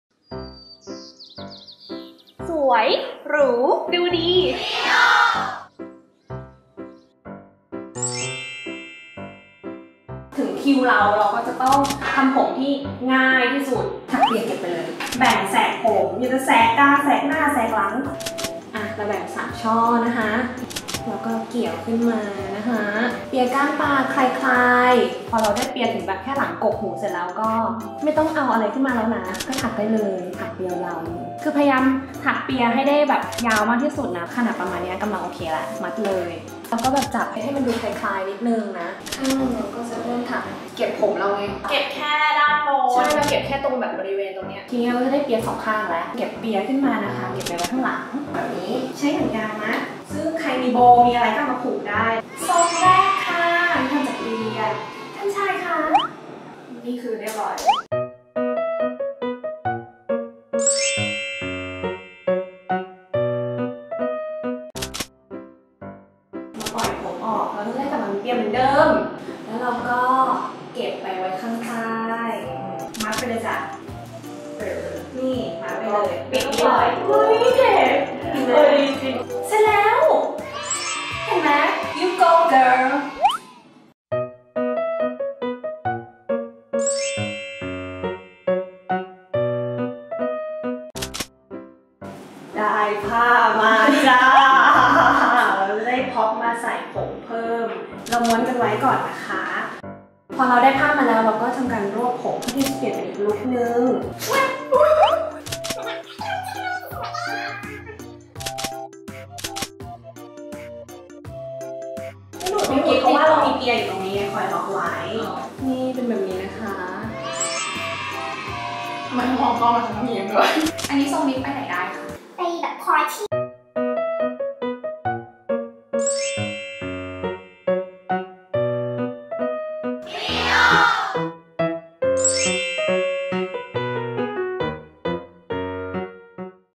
CLEO LET'S DO HAIR EP03 ถักเปียรอบเดียว ได้สวยสามลุค
❤️❤️❤️คลิป how to ทำผมอันนี้มีมากกว่า 1 ลุค ไม่น่าเชื่อ่วาผมทรงเดียวจะสามารถปรับเป็นทรงใหม่ได้อีกตั้ง 2 ทรง ที่สำคัญทำง่ายเว่อร์ 😘😘😘